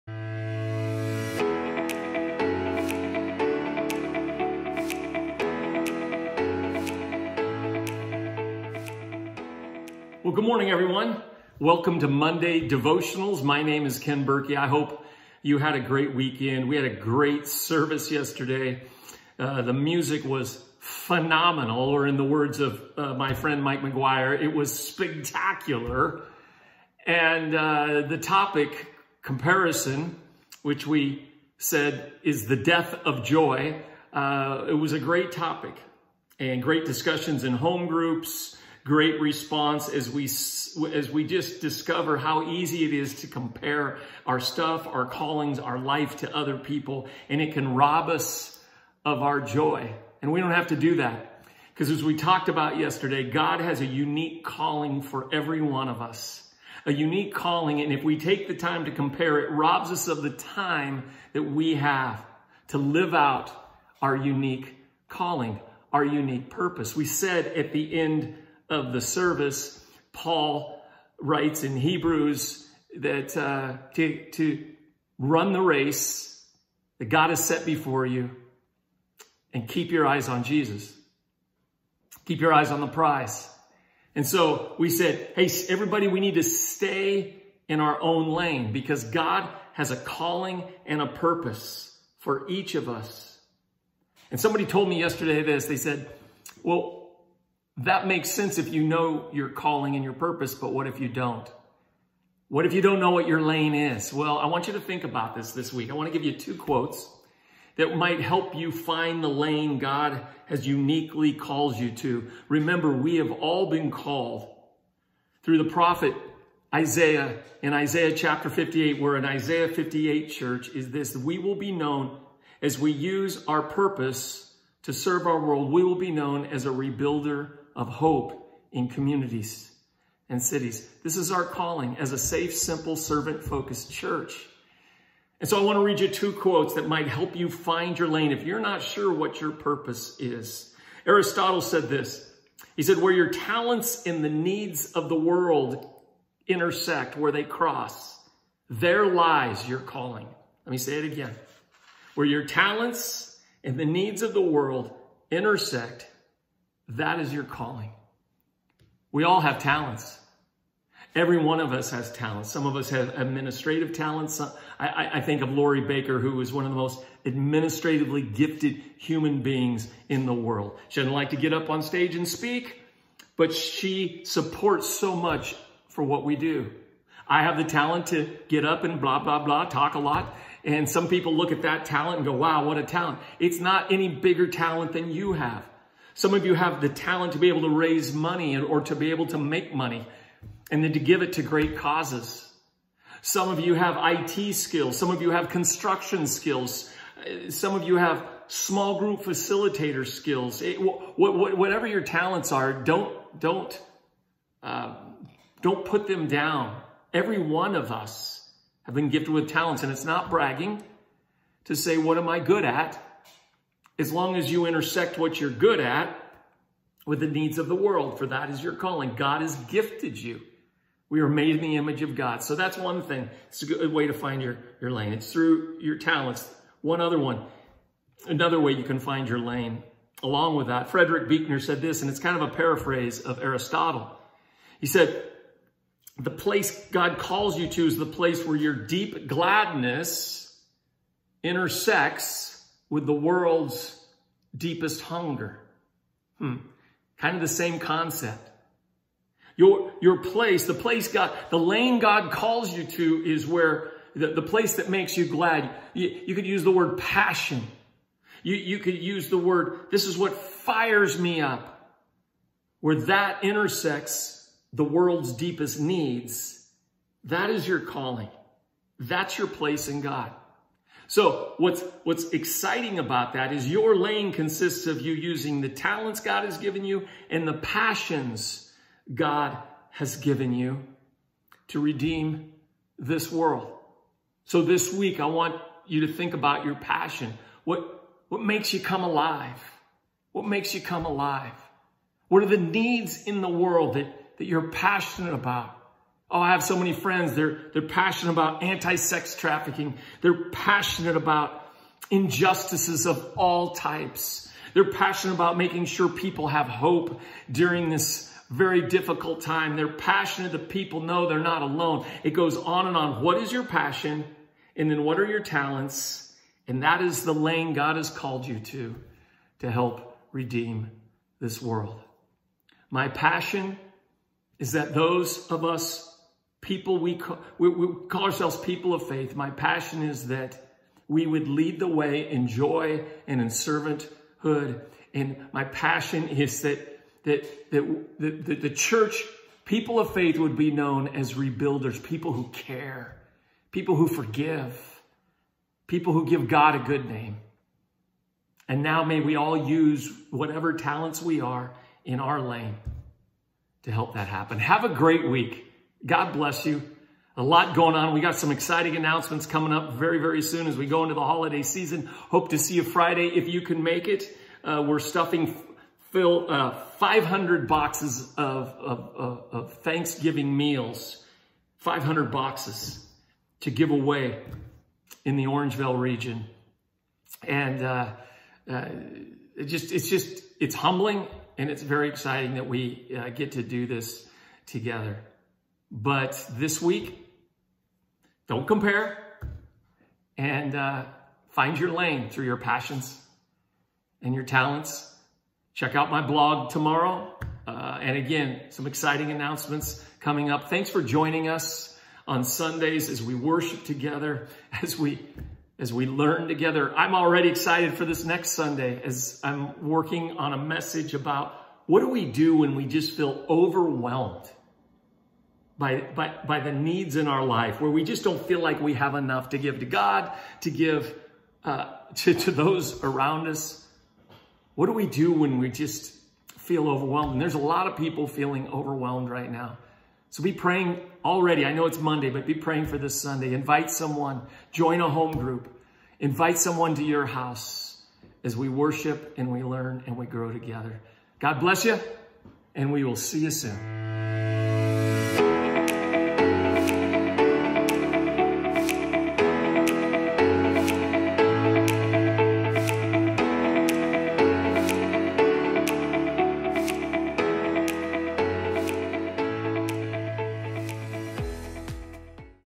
Monday Devotional "Where your passions and the world's needs collide" Ken Burkey
Monday Devotional with Live58 Church! - "Where your passions and the world's needs collide" - Ken Burkey